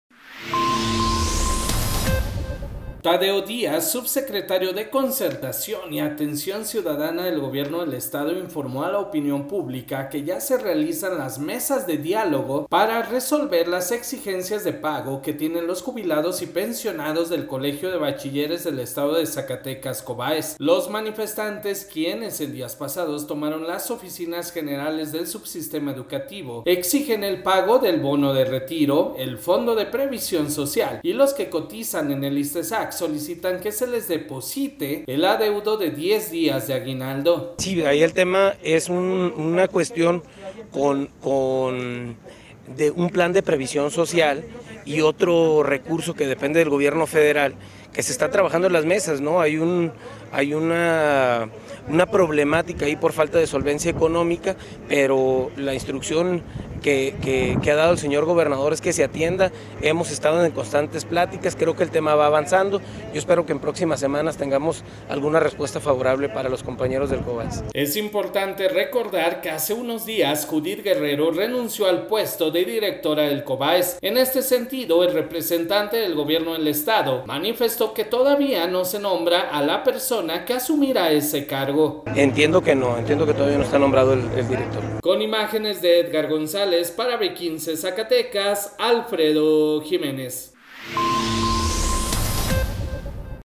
Atiende el Gobierno del Estado a los jubilados y pensionados del Cobaez
Tadeo Díaz, subsecretario de Concertación y Atención Ciudadana del Gobierno del Estado informó a la opinión pública que ya se realizan las mesas de diálogo para resolver las exigencias de pago que tienen los jubilados y pensionados del Colegio de Bachilleres del Estado de Zacatecas (Cobaez), los manifestantes quienes en días pasados tomaron las oficinas generales del subsistema educativo exigen el pago del bono de retiro, el fondo de previsión social y los que cotizan en el Issstezac solicitan que se les deposite el adeudo de 10 días de aguinaldo.

Es importante recordar que hace unos días Judit Guerrero renunció al puesto de directora del Cobaez, en este sentido, el representante del Gobierno del Estado manifestó que todavía no se nombra a la persona que asumirá ese cargo.